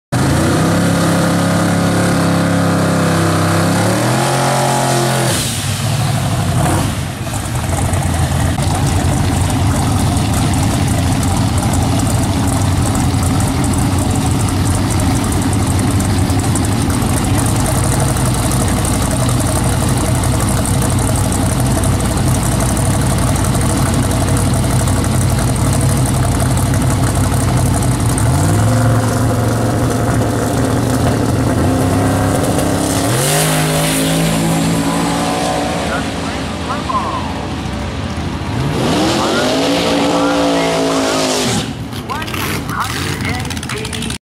Vulcan "Big V" Mclellan goes 7.86 at 180 in the twin turbo camaro at Woodburn!
Vulcan "Big V" Mclellan goes 7.86 at 180 in the twin turbo camaro.